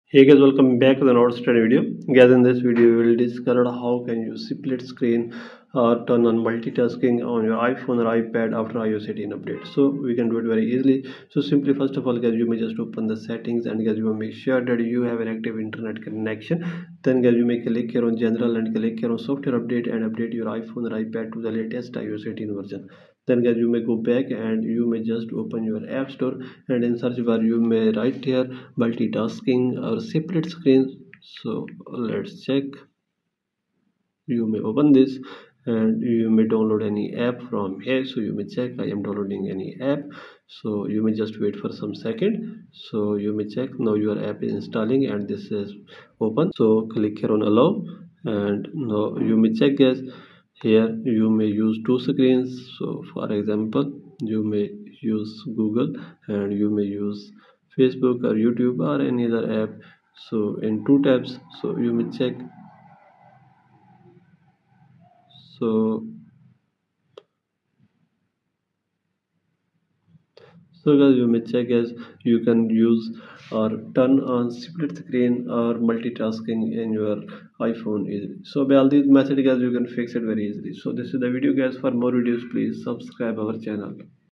iOS 18 Split Screen: How To Use Split Screen Feature on iPhone X/ Xs/11/12/13/14/15/16 | New Update
Want to multitask on your iPhone with the split-screen feature in iOS 18? In this video, I’ll show you how to use the split-screen or multitasking feature on iPhone models like the X, Xs, 11, 12, 13, 14, 15, and 16. Easily run two apps side by side and boost your productivity!

What You'll Learn:

How to enable split-screen on iPhone

How to switch between apps while multitasking

Tips for making the most of iOS 18's multitasking features